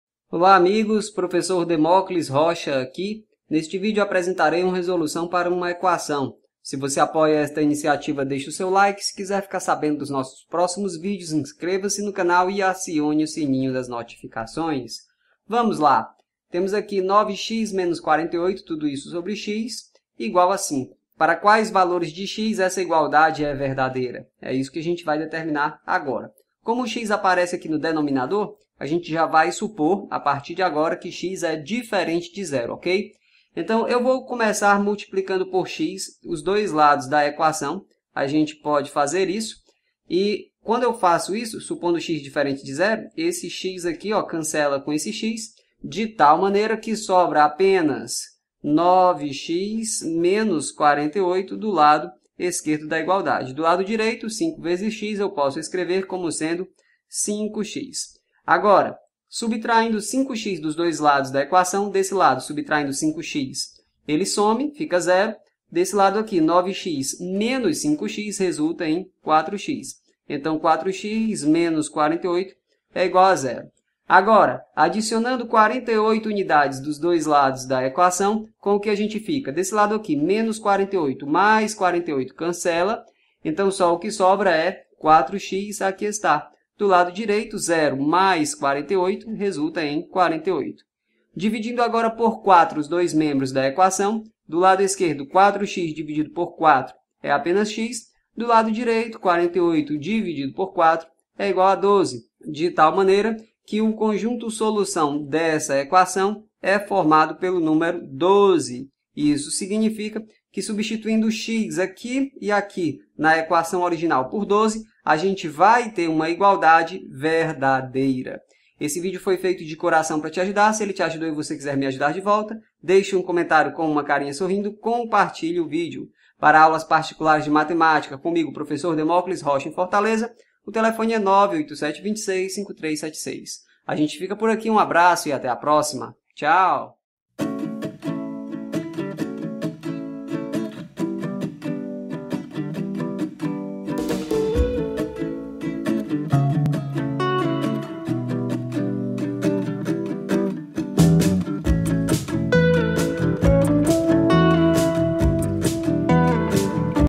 EQUAÇÃO
Olá, amigos do meu canal no YouTube. Eu sou o professor Demóclis Rocha e, neste vídeo, apresentarei uma resolução para uma equação.

CÂMERAS

MICROFONES